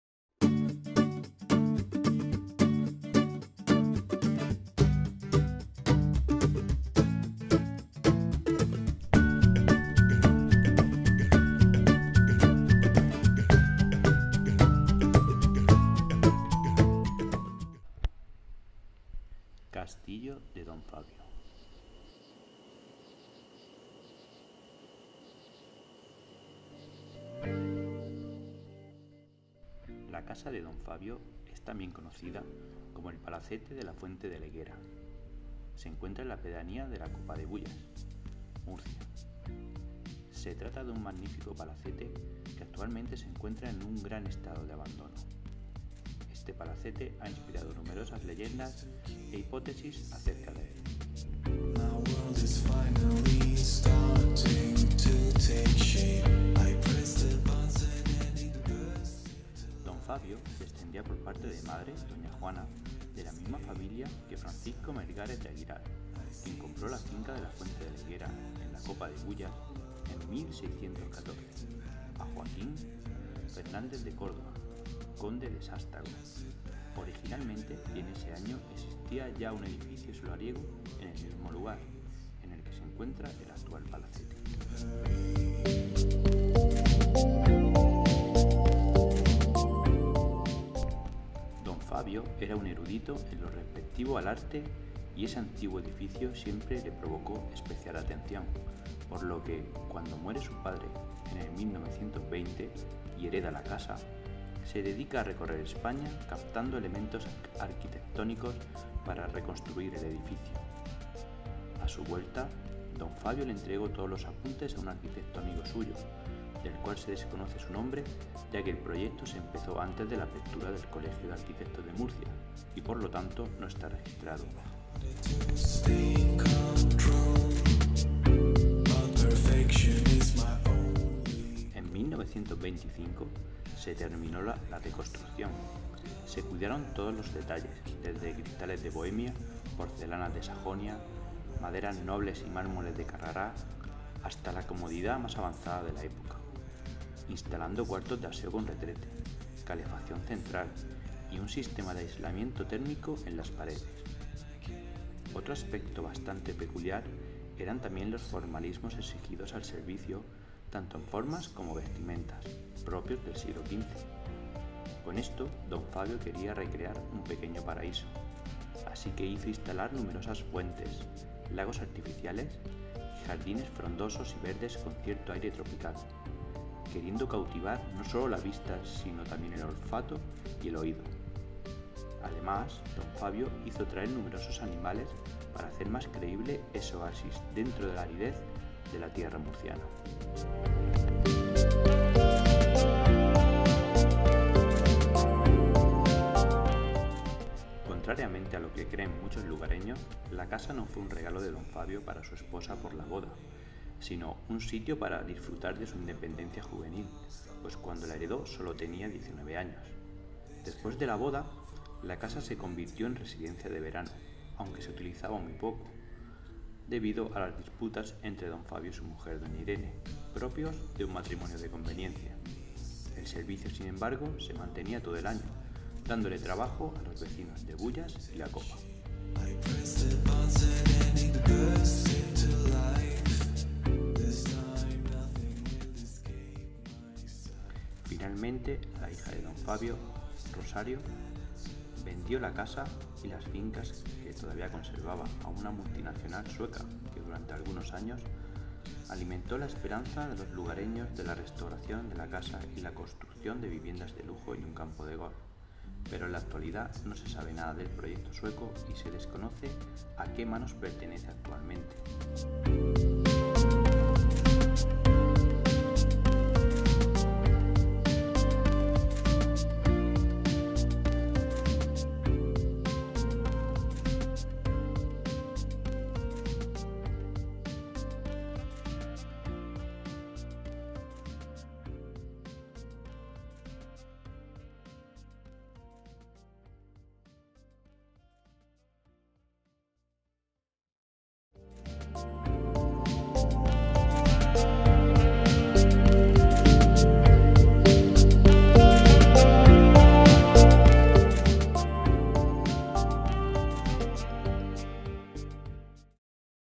Castillo Don Fabio. La Copa de Bullas
Aquí os dejo unas vistas aéreas del Castillo de Don Fabio y un breve audio de la historia del mismo.

Como siempre, espero k os guste, y agradezco k lo compartáis si os gusta!!